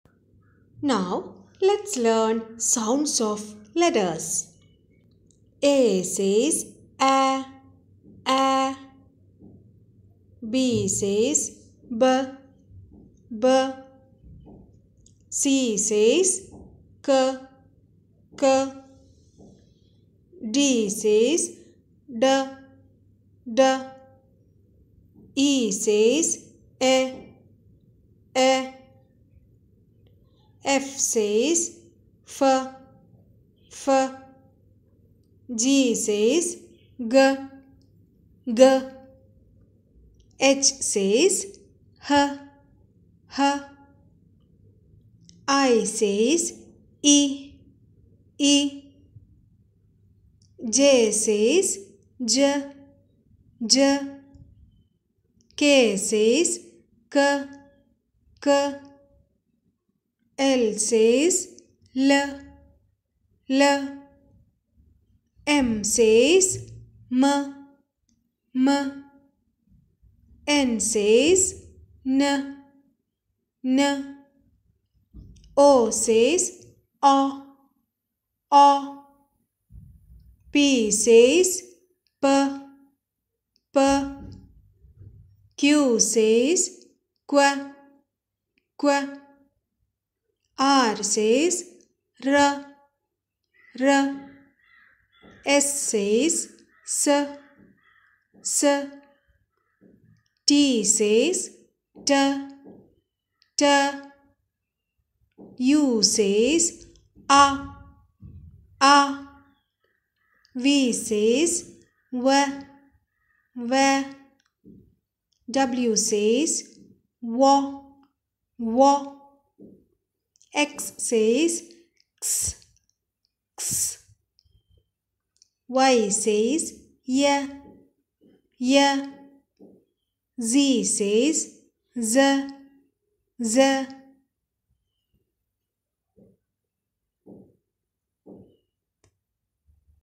The sounds of alphabets l Learn Phonics l sounds of letters for kids
The letter sounds for preschool children.

Pearls Land
PearlsLand

You Tube
You Tube Videos